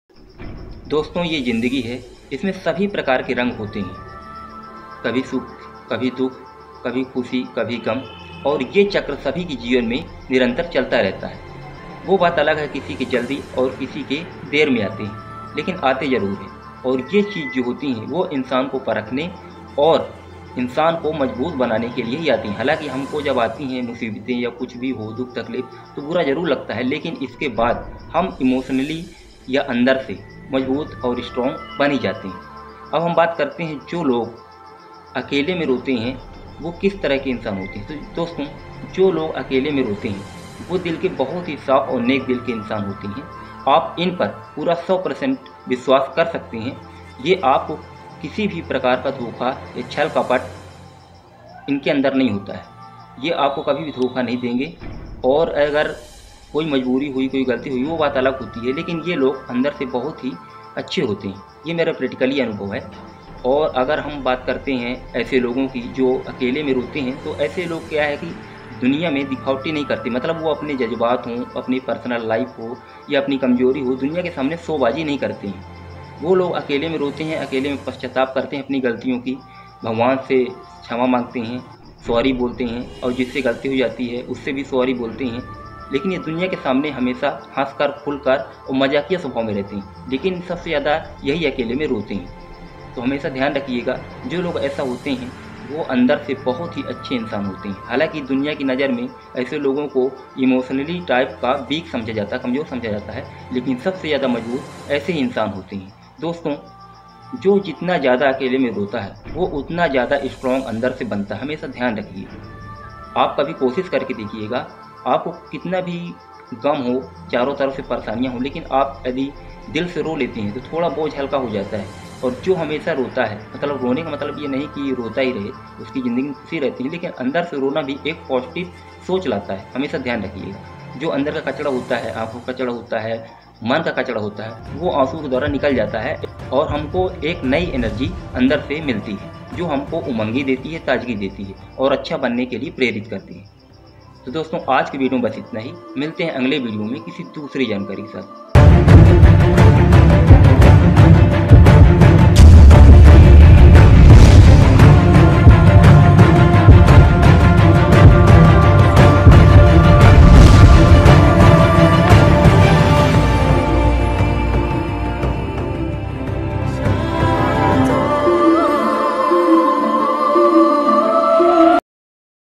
अकेले में खुद से रोने वाले इंसान अंदर से कैसे होते है ? who cry themselves alone come from inside?
How do humans who cry themselves alone come from inside?
जो इंजन दिल से अकेले में खुद से रोते है वो अंदर से कैसे होते है दूसरो के लिए